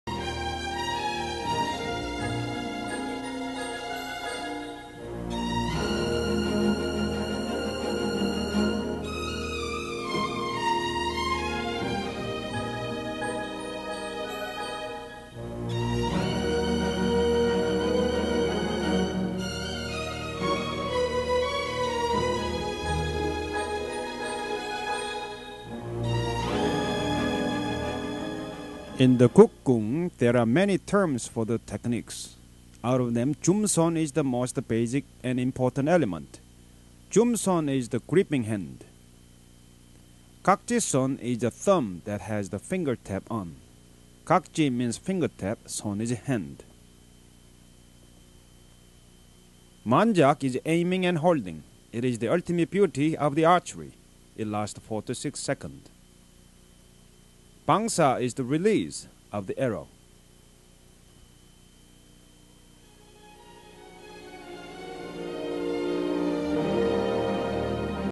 Korean Archery (traditional) Terminology
Learn the Korean and English terms used in the practice of Korean Traditional Archery (Kuk Kung, Gong Sul or Goong Do). For more traditional archery on YouTube check out the archer playlist